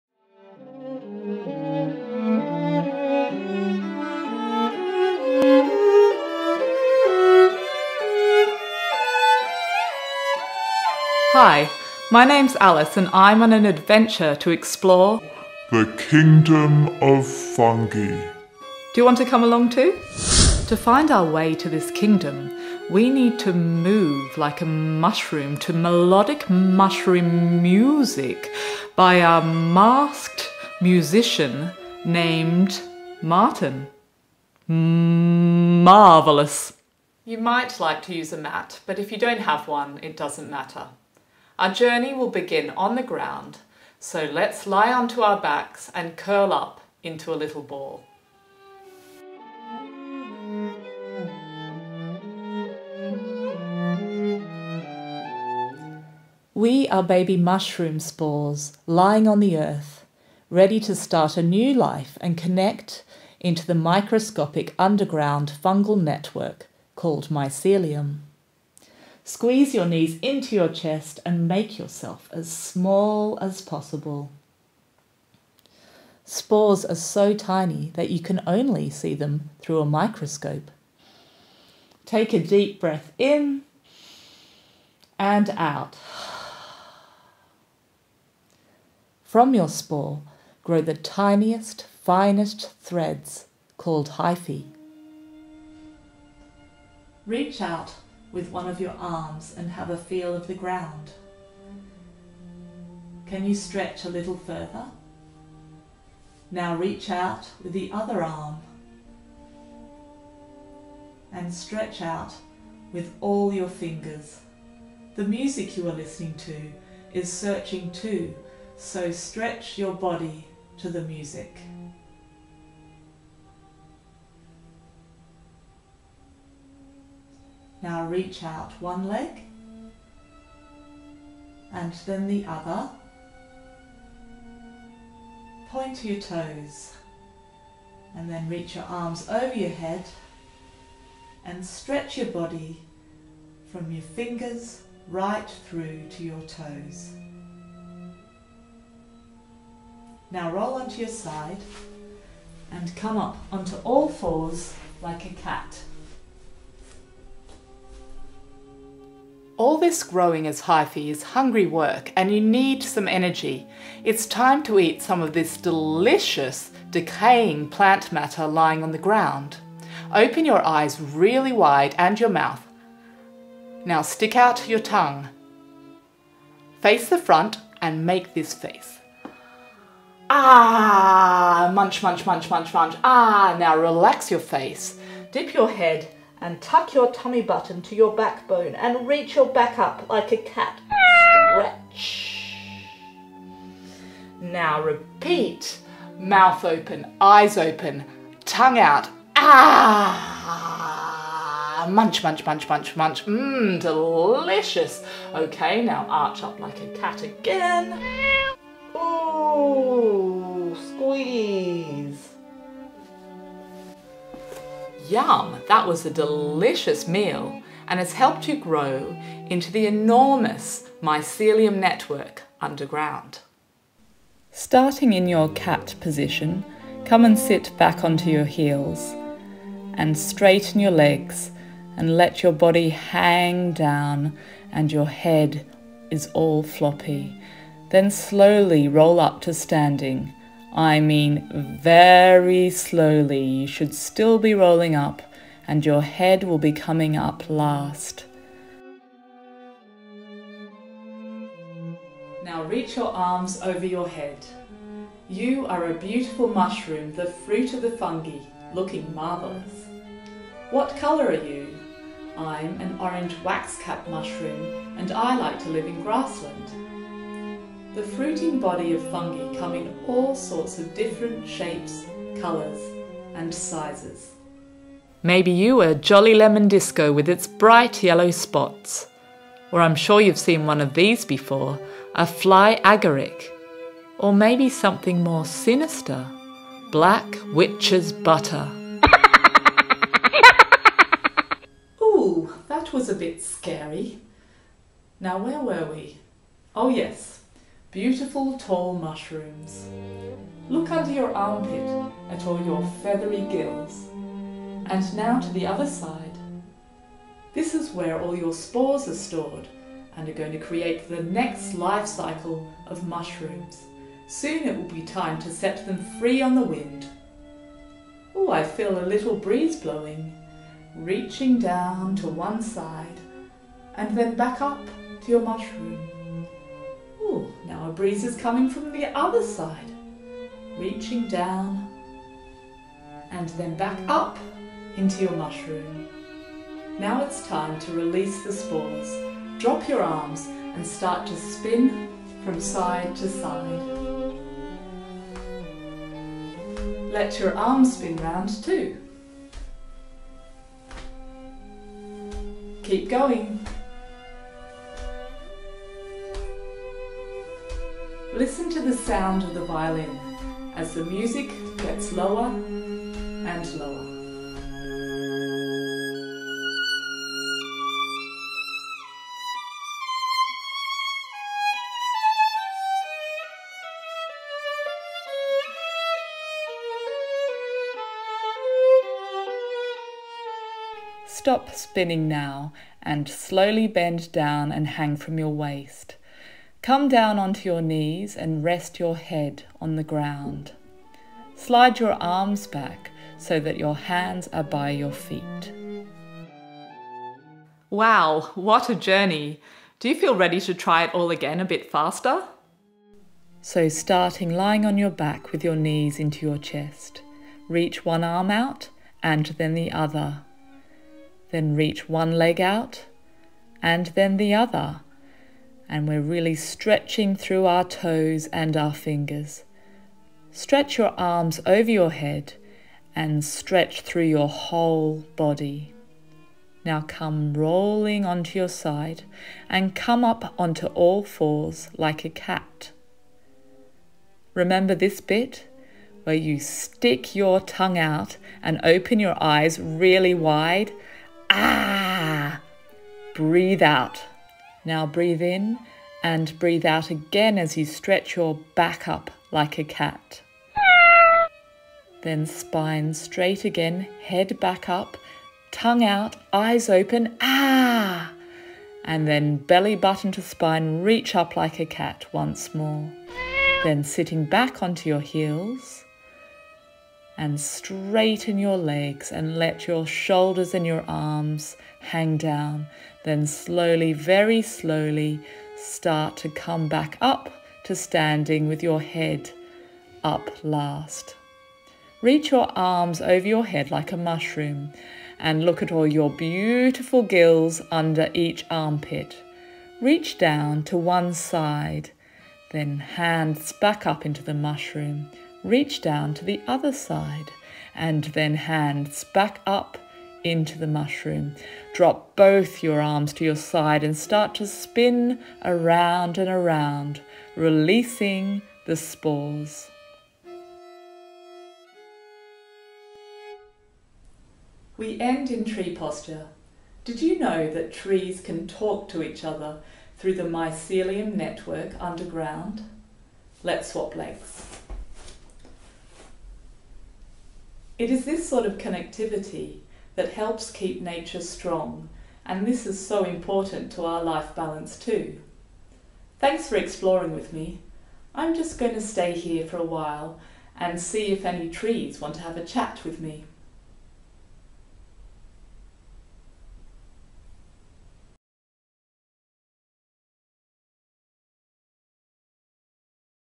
Move like a Mushroom - TRANSPLANTED Carbon (yoga for kids)
This quirky yoga sequence follows the life cycle of a mushroom.

Created by SEQUOIA – Alice Rickards and Sonia Cromarty

Lemon Disco © Tatiana Bulyonkova/Plantlife

Black Witches Butter © Ray Woods/Plantlife

Oak © Polly Phillpot/Plantlife

Thanks to Chamber Music Scotland